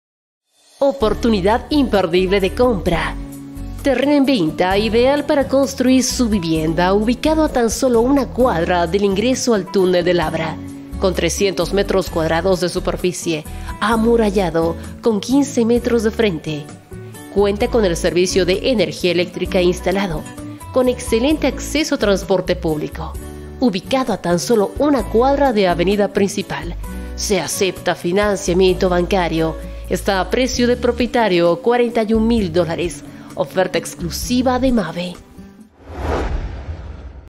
Cochabamba, Zona el Abra a 1 cuadra de Av. Principal
Lote en venta, Cochabamba, Zona el Abra a 1 cuadra de Av. Principal, sup. ideal de  300 M2, amplio frente de 15 mtrs, servicios básicos disponibles incluido el gas domiciliario, semi amurallado, papeles al día.
💰 Precio: $us 41.000.-
¡Hablemos por WhatsApp!  ✆ 61680280 ó
🎯 Oferta Exclusiva de MABE®
Of: Av. Belzu Nº 160 📍
🇧🇴 Cochabamba - Bolivia